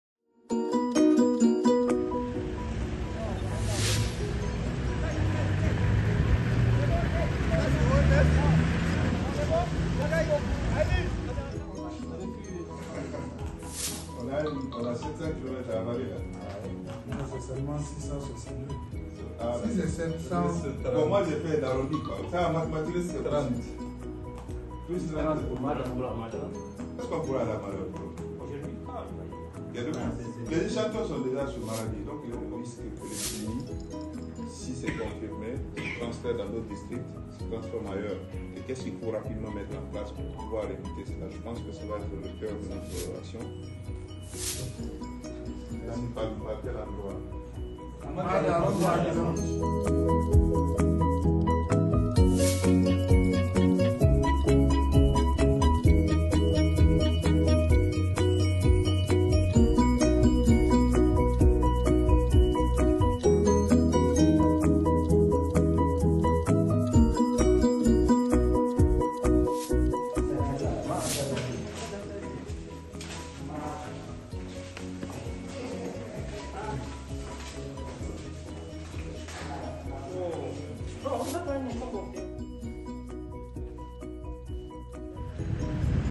Déploiement SURGE dans les régions de Maradi et Zinder riposte épidémie de choléra
Déploiement de l’équipe SURGE dans les régions de Maradi et Zinder en appui à la riposte à l’épidémie de choléra. A la fin du mois d’août 2022, le Niger a connu une nouvelle épidémie de choléra qui a démarré de façon concomitante dans les régions de Maradi (district de Madarounfa) et de Zinder (district de Dungass). Pour répondre à cette épidémie, l’OMS a soutenu le déploiement rapide dans les deux régions de l’équipe SURGE « Renforcement et utilisation des groupes de riposte aux situations d’urgence » formée pour appuyer à temps la réponse aux situations d’urgence. Ainsi, en 48 heures, des équipes multidisciplinaires d’experts nationaux étaient présentes sur le terrain pour coordonner la réponse d’urgence, y compris le laboratoire mobile déployé dans la région de Maradi pour appuyer la confirmation rapide des cas.